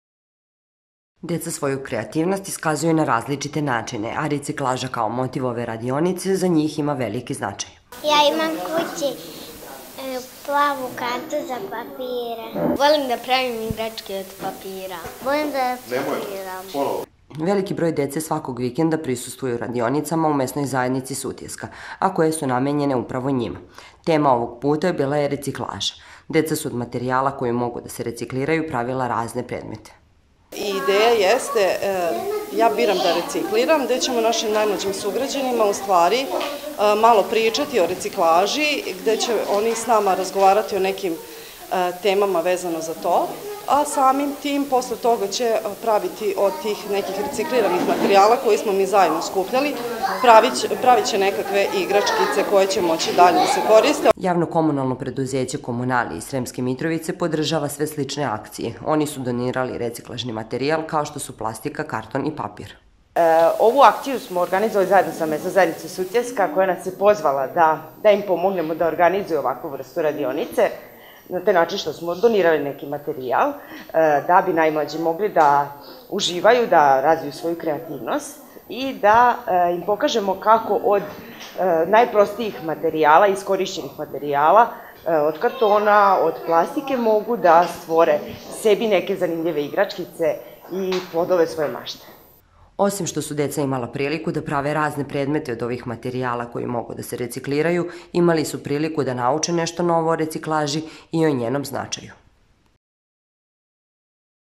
“Ja biram da recikliram”
Dečja ekološka radionica “Ja biram da recikliram” koja je nedavno održana u Mesnoj zajednici Sutjeska u Sremskoj Mitrovici samo je jedna u nizu aktivnosti koje se organizuju svakog vikenda u ovoj Mesnoj zajednci. Realizaciju ove radionice pomoglo je Javno-komunalno preduzeće “Komunalije” koje je doniralo reciklažni materijal od kog su deca pravila predmete i igračke.